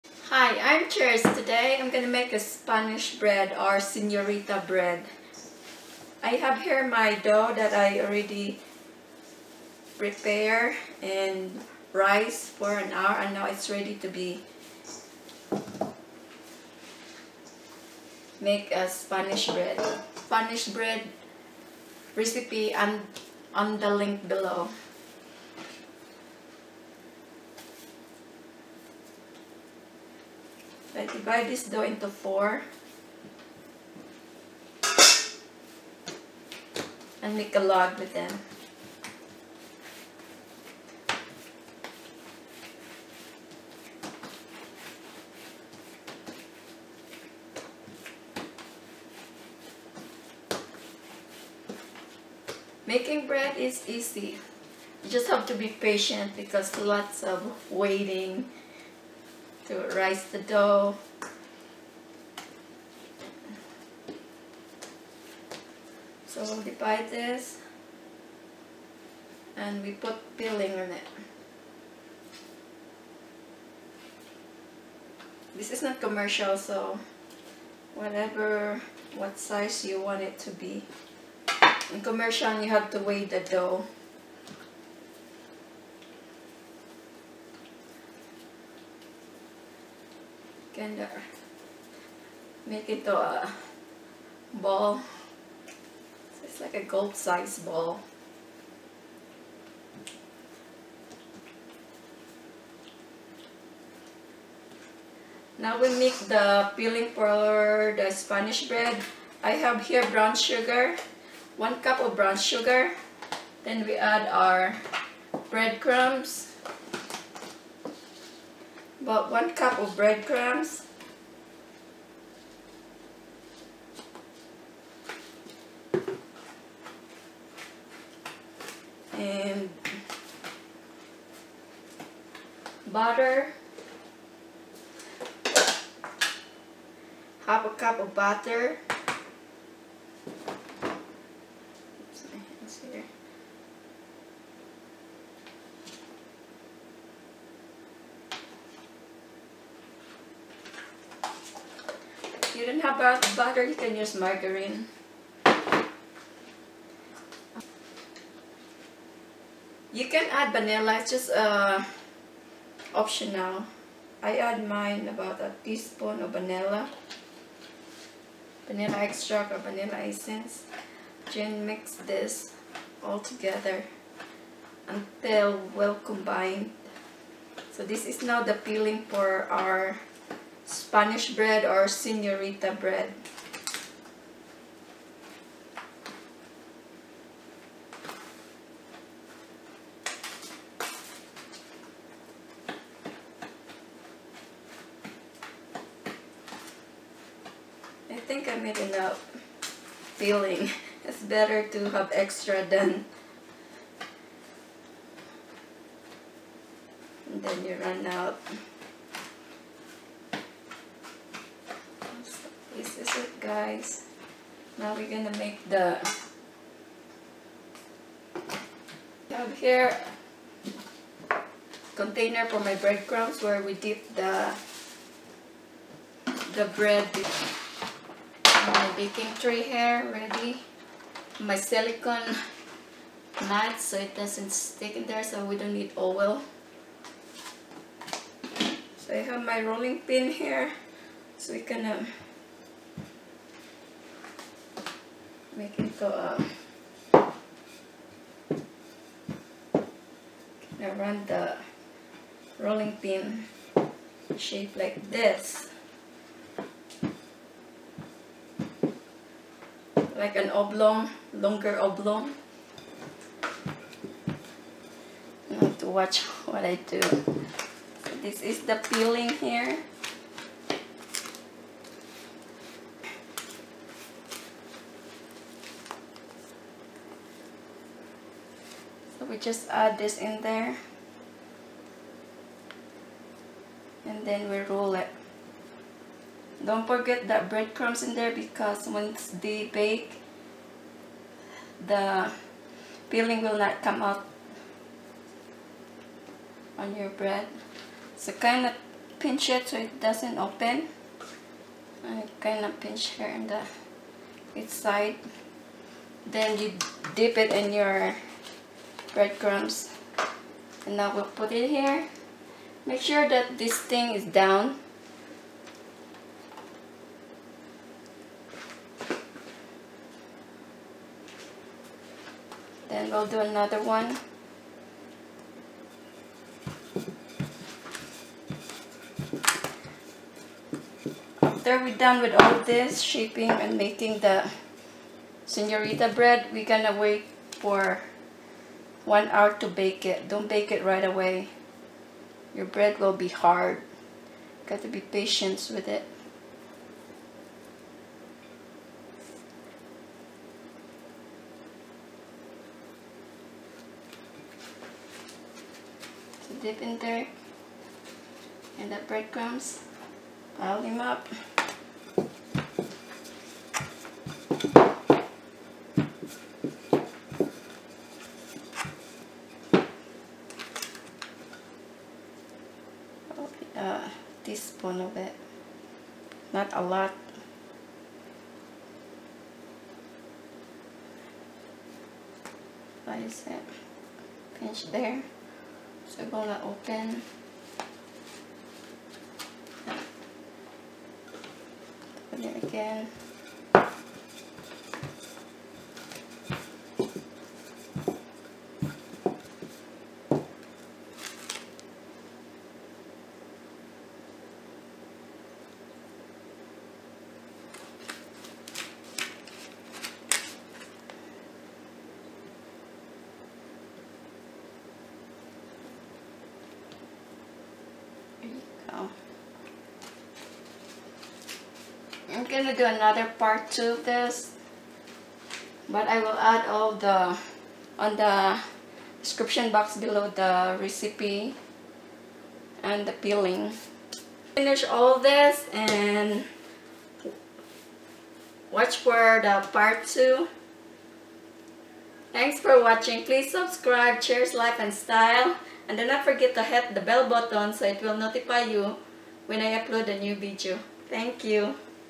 Spanish Bread or Senorita Bread recipe. Yummy! Part 1 of 2
Spanish or Senorita Bread
Ingredients:
1 and 1/4 cups warm milk
3  tsp active dry yeast
4 cups All purpose Flour
1 tsp salt
1/2 cup sugar
1/3 cup canola oil or margarine
1 large egg lightly beaten
3/4 cup bread crumbs for coating the bread
1/4 cup evaporated milk

For the filling:
1 cup Brown sugar
1 cup Breadcrumbs
1/2 cup butter
1 tsp vanilla
Mix all the ingredients until well combined. Its ready.

Coating: Breadcrumbs

Procedure Dough
Make the Bread Dough
In a mixing bowl, combine milk and yeast. Let stand for 5 minutes or until foamy. Add the eggs, sugar, oil and salt. Stir everything with a wooden spoon. Add 4 cups of flour, one cup at a time while stirring. The mixture will turn into a shaggy dough.

Sprinkle just enough of the remaining 1/2 cup of flour and keep stirring until the dough gathers in the center. Turn the dough over on a floured board and knead it until it is smooth and elastic, about 6-10 minutes. As you knead, dust your hands and your board as needed to help with the stickiness. You may need to use up to 1/8 cup of additional flour.

Shape the dough into a ball and place it a bowl. Cover and let rise for 1 and 1/2 hours, or until the size is doubled. You can make the filling at this point.
Put the filling then wait for 1 hour to rise. Bake 375 F for 12 to 15 minutes.